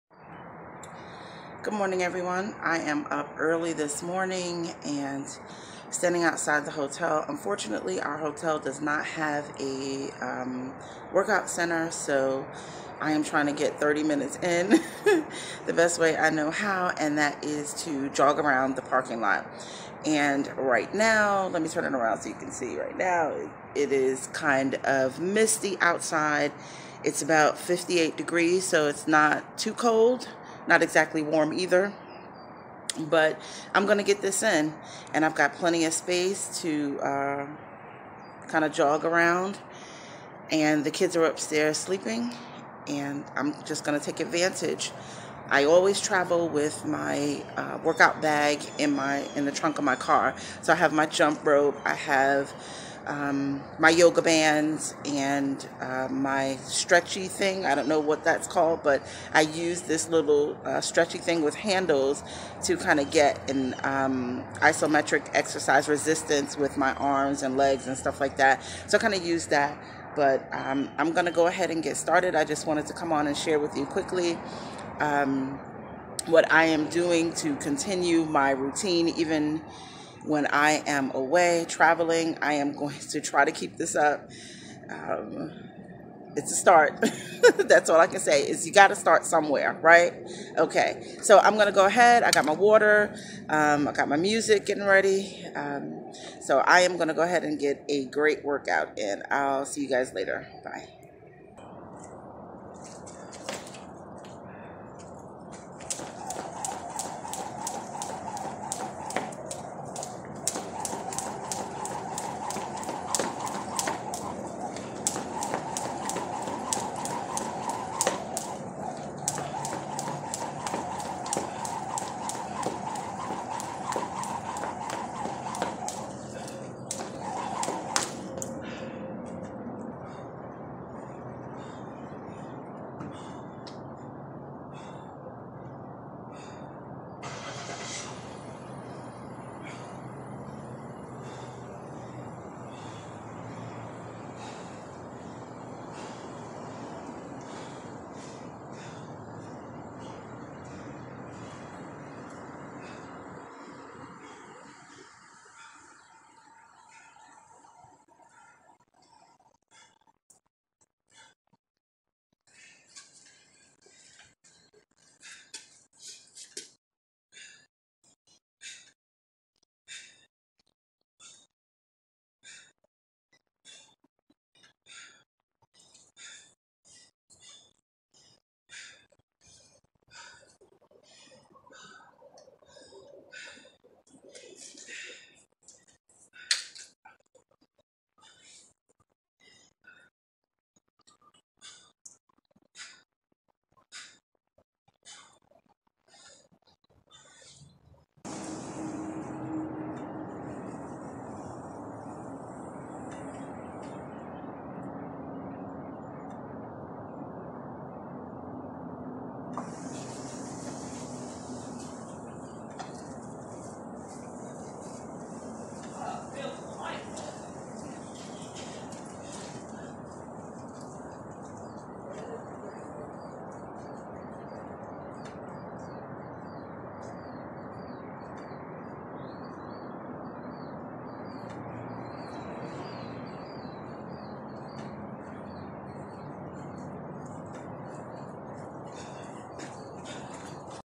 Parking Lot Workout
How I workout when there's no indoor facility